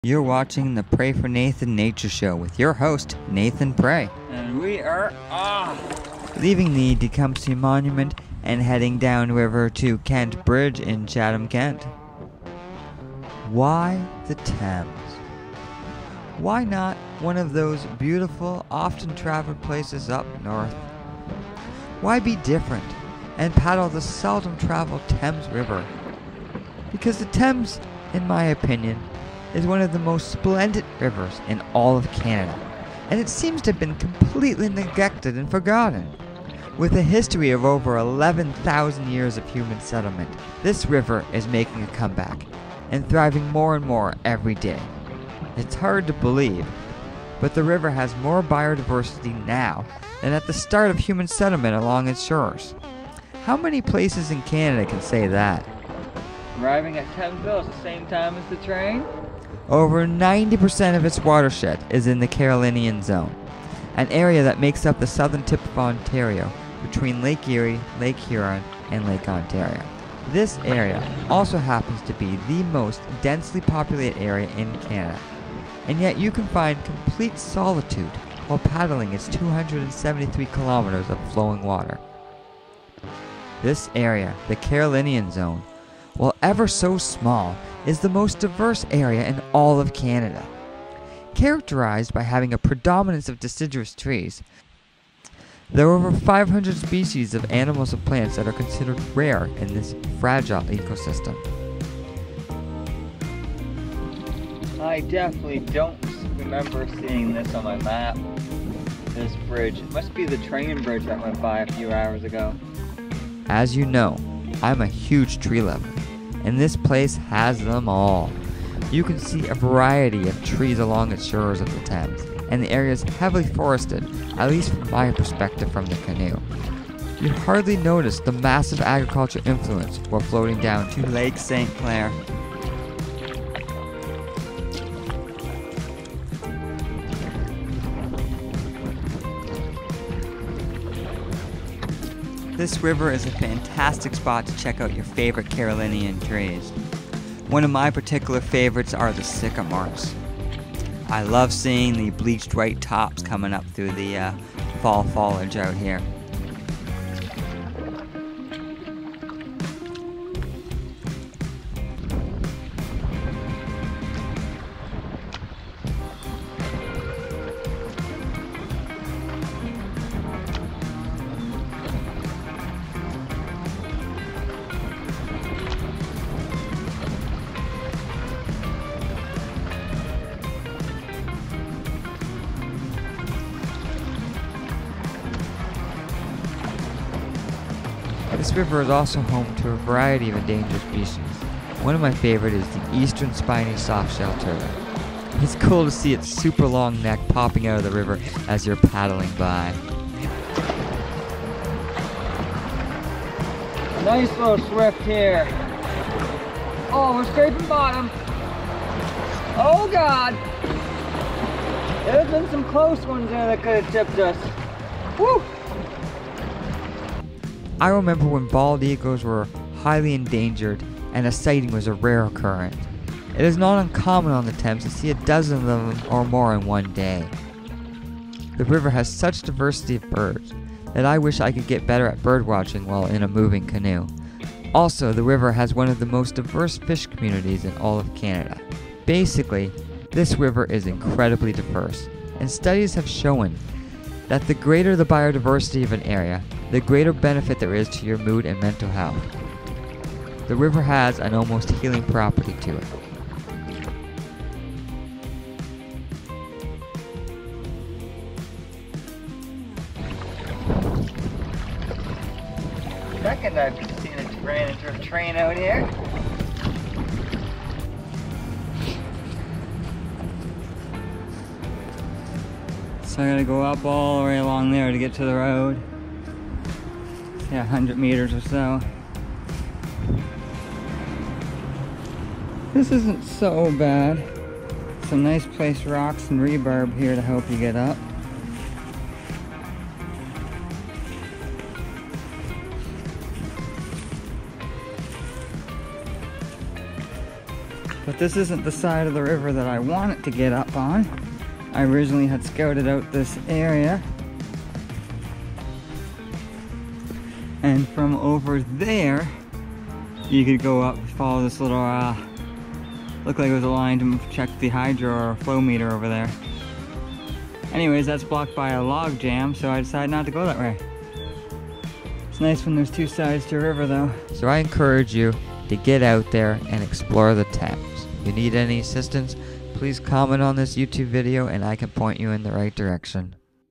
Why you should Paddle the Thames River
The Thames River is the best river in Canada, yet hardly anyone is paddling it.  This is a trip from the Tecumseh Monument to Kent Bridge with a backdrop of fall colours on Canada’s most diverse river.